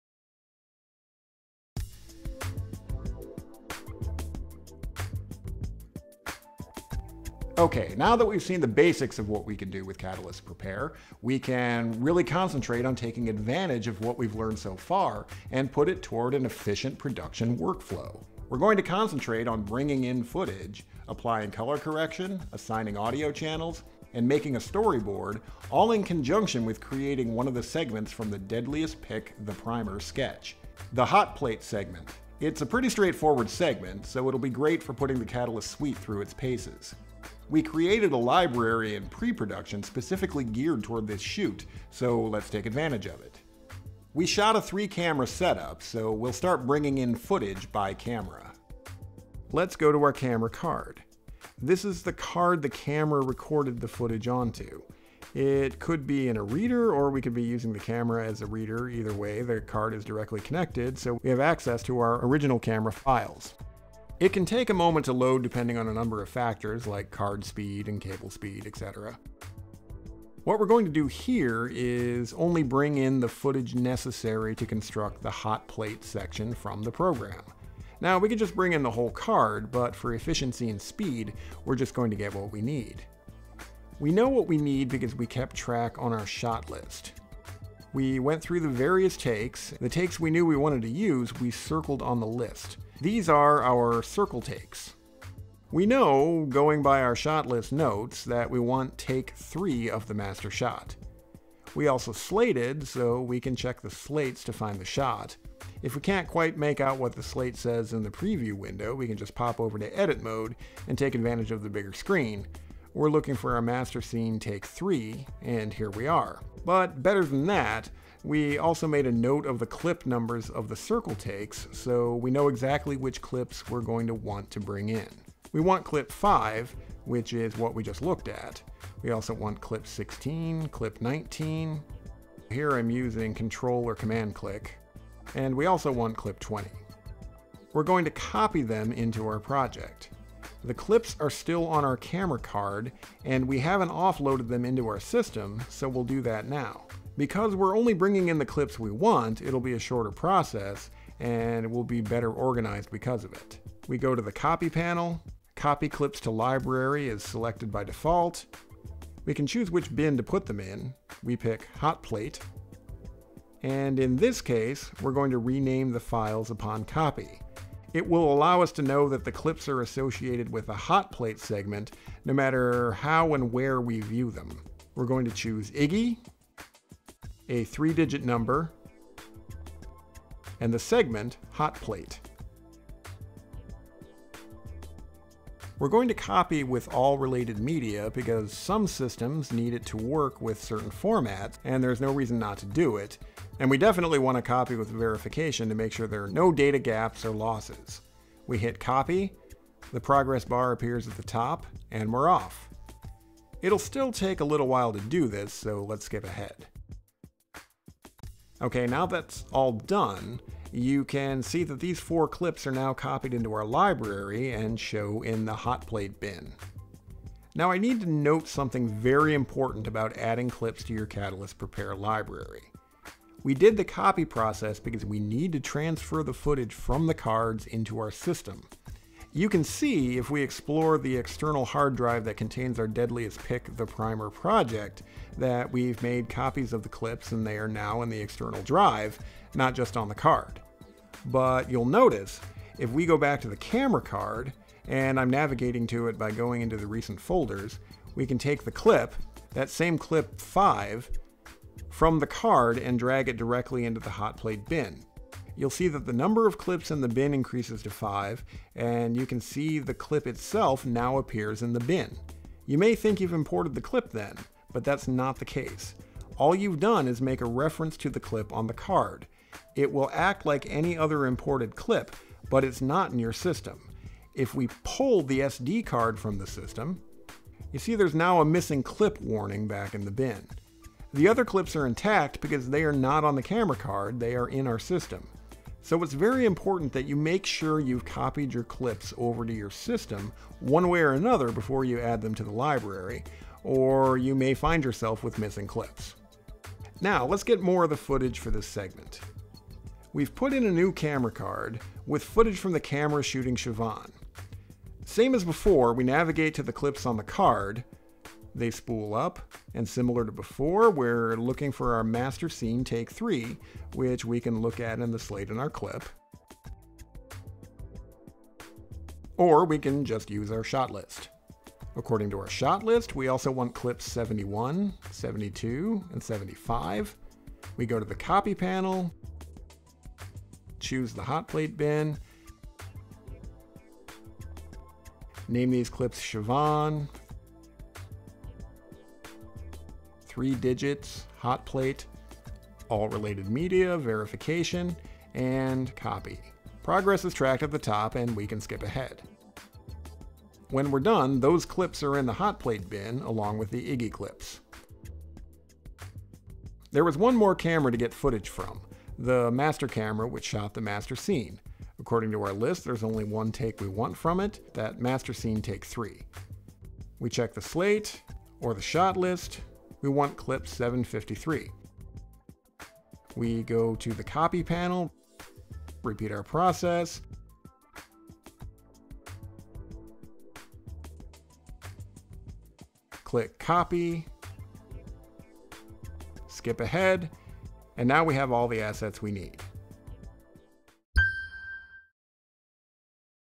Advanced Importing Functionality in Catalyst Production Suite
An introduction to advanced importing in Catalyst Production Suite, produced by David Jimerson of Wrightsville Beach Studios